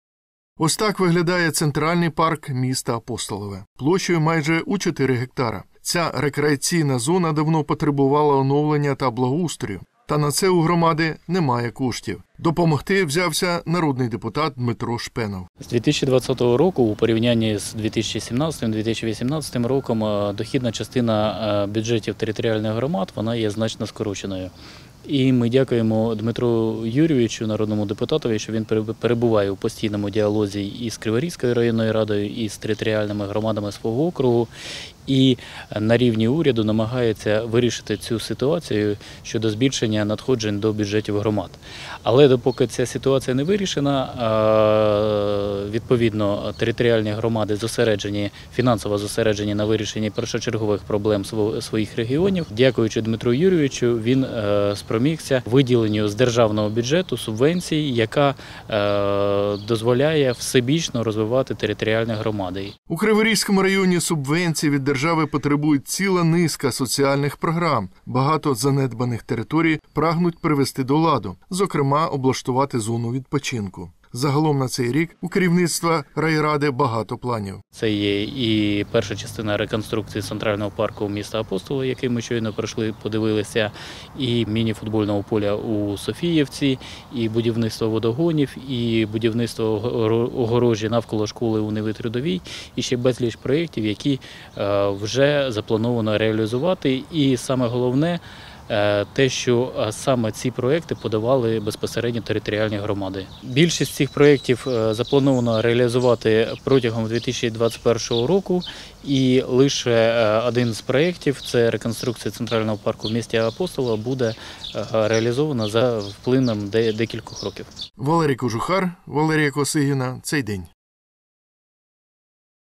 Для розвитку громад: в Криворізькому районі реалізують соціальні проєкти
Кабмін провів розподіл державних субвенцій для місцевих бюджетів на реалізацію заходів щодо соціально-економічного розвитку окремих територій, зокрема і у Дніпропетровській області. У Криворізькому районі коштом держсубвенції планують реалізувати цілий ряд проєктів у громадах.